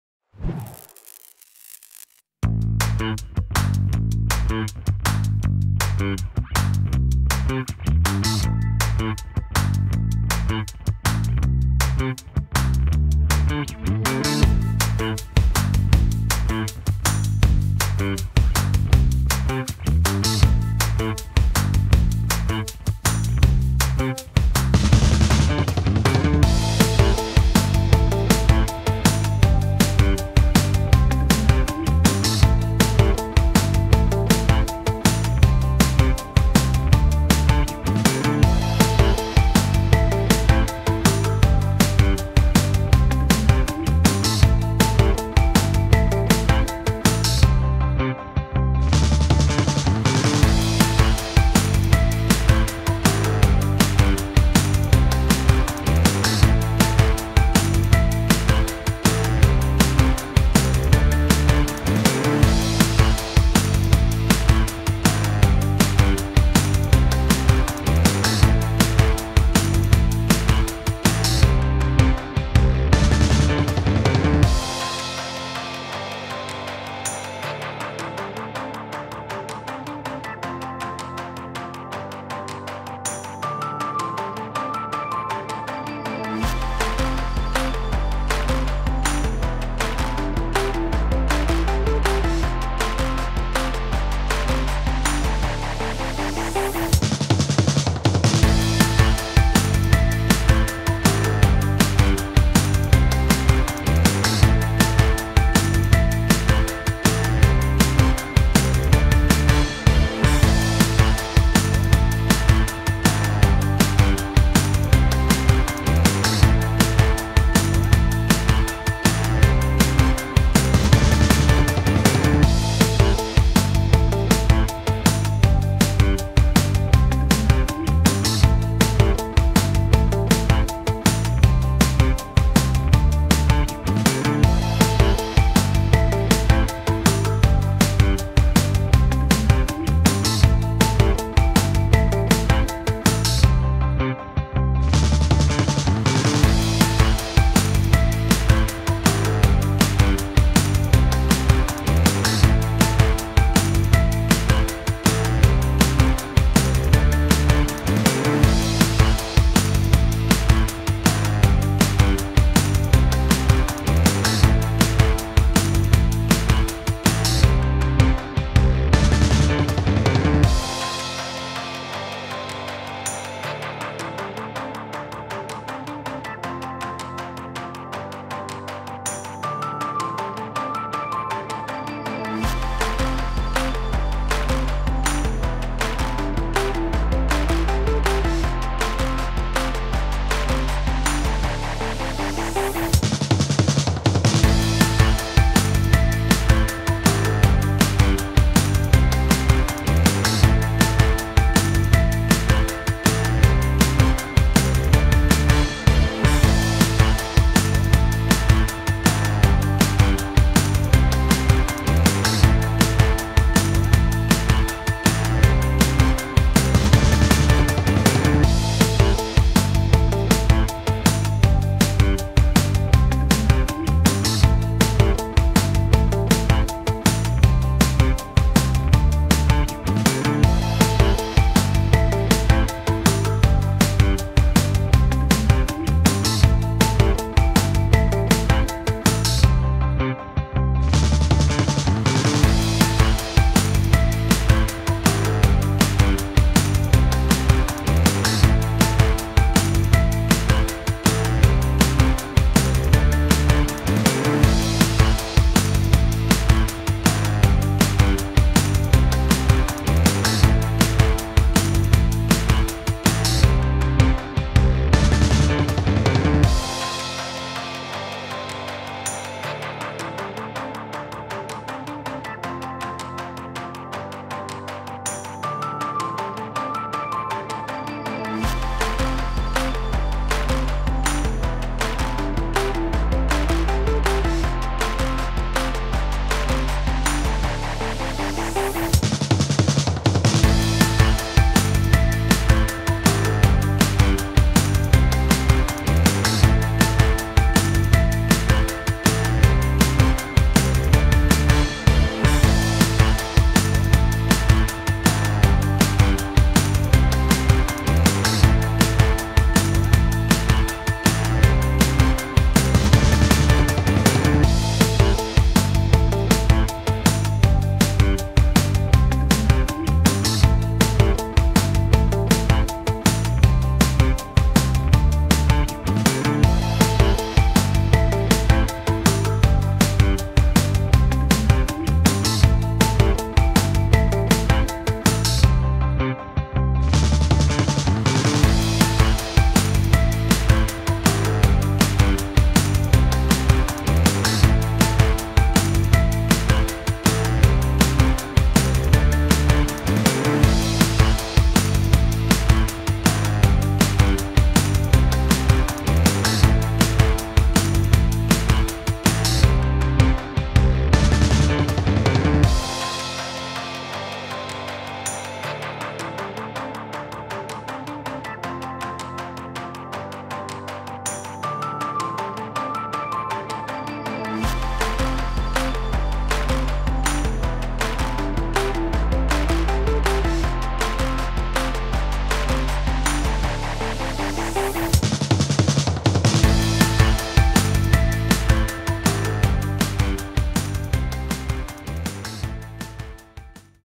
Assembling the Xootz Comet Electric Go Kart
Take a look at how to assemble the Xootz Comet Electric Go Kart.